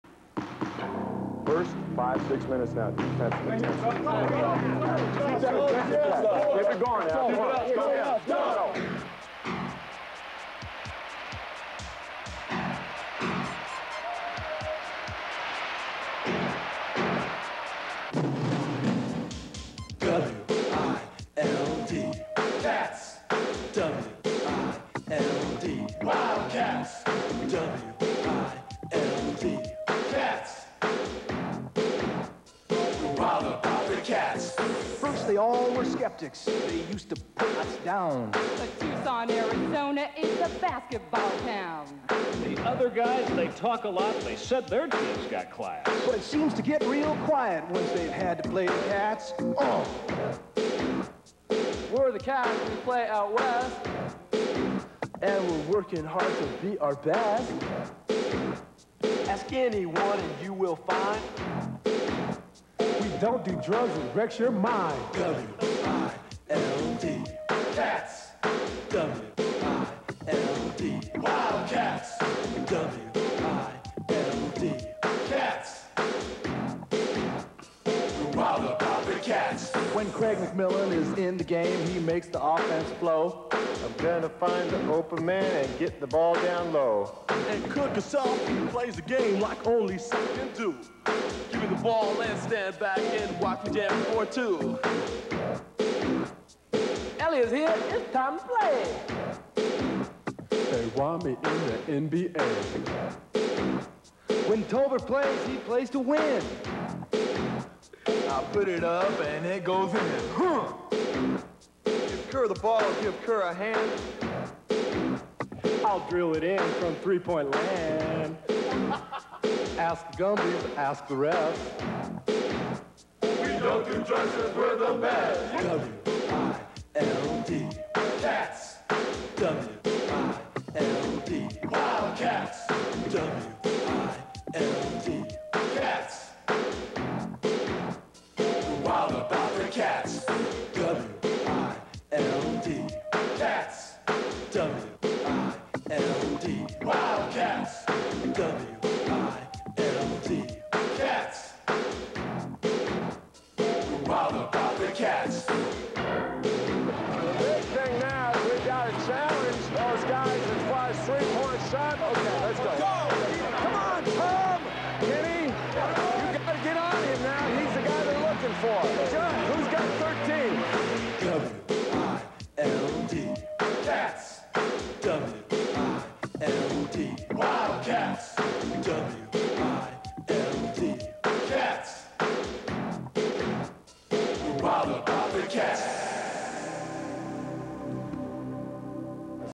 Arizona Wildcats Basketball - Wild About the Cats (1987-88)
Video made by the Arizona Wildcats basketball team in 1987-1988.  Features Sean Elliott, Steve Kerr, Anthony Cook, Tom Tolbert, Craig McMillan, Harvey Mason, Kenny Lofton, Jud Buechler, Sean Rooks, Brian David, Craig Bergman, Joe Turner, Matt Muehlebach and Lute Olson.